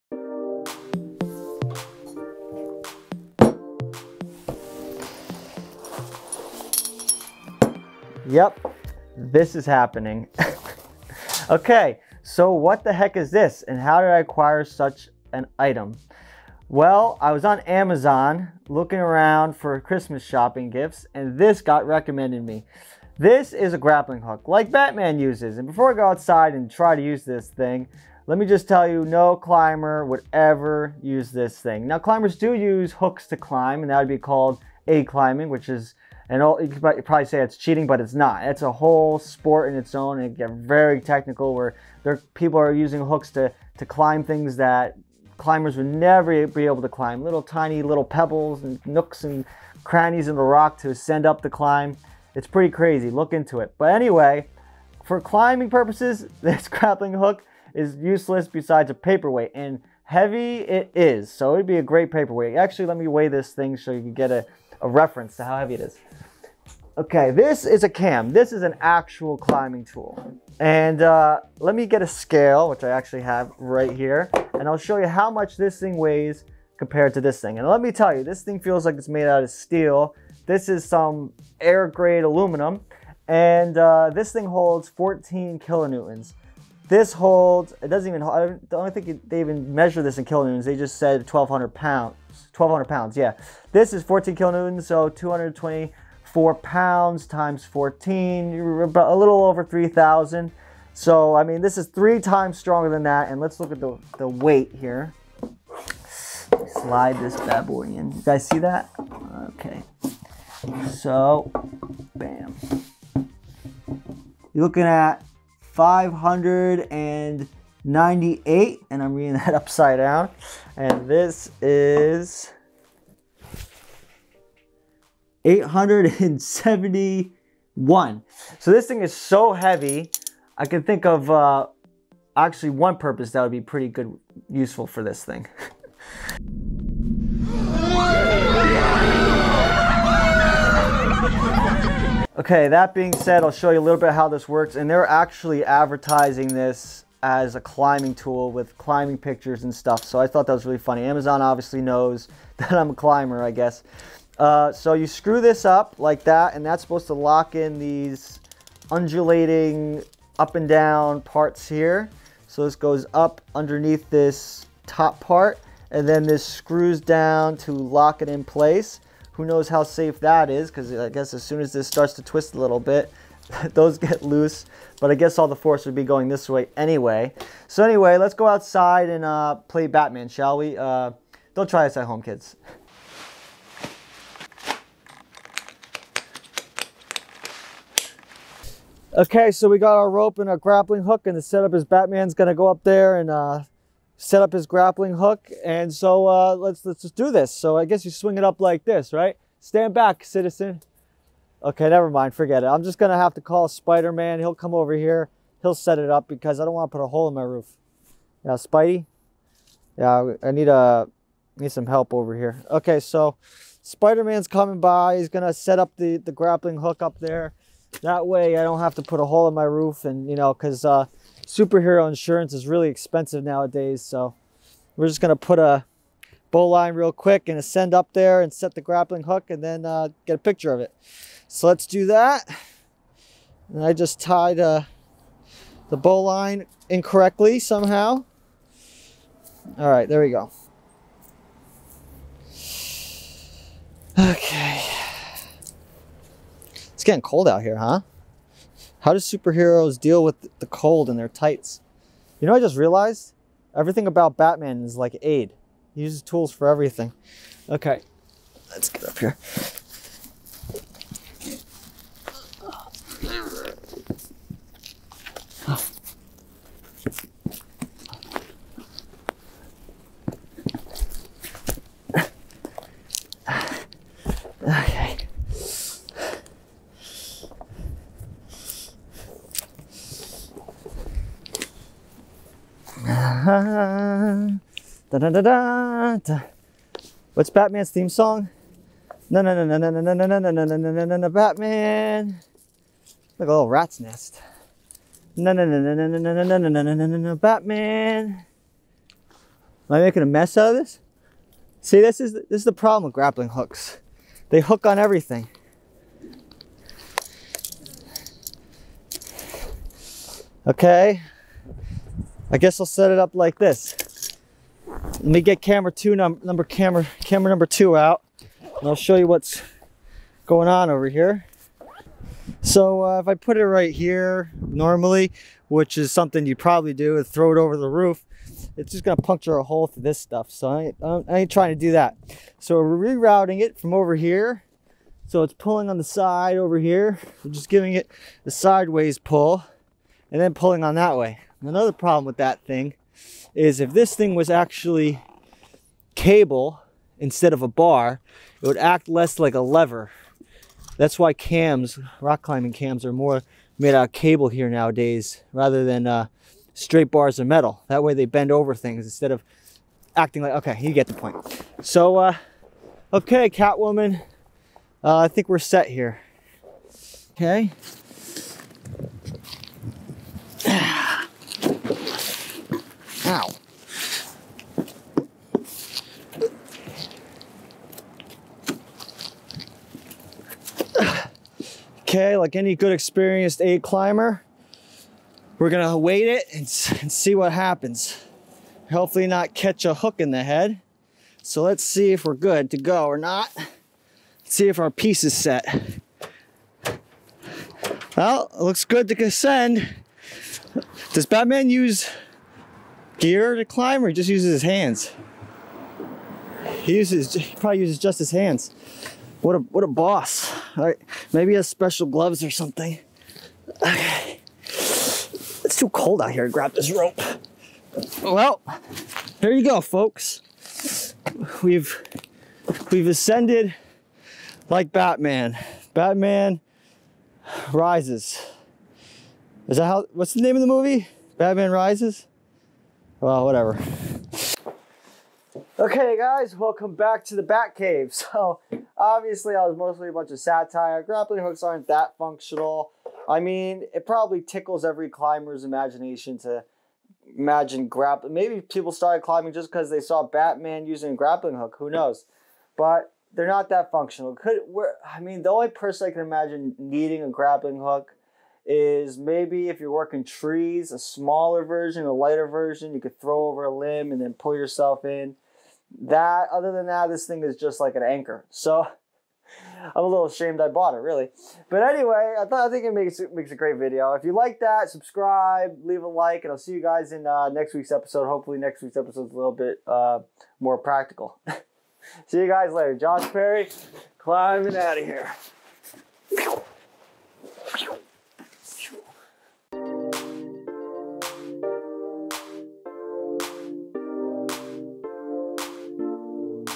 Can you really use a Grappling Hook like Batman?!
Hey guys look what I found on Amazon! Yepp it is the most useful climbing tool in the crime fighting biz. Yeah.. so in this episode I show that grappling hooks aren't as useful as they may seem for climbing. You would have to get very lucky to get a good placement with one, and even if you did, chances are you would never know what it looked like till you got to it. I guess it makes for great TV though. Sorry for destroying your child hood fantasies with this one guys!

Here is a Discord link, come join the fun!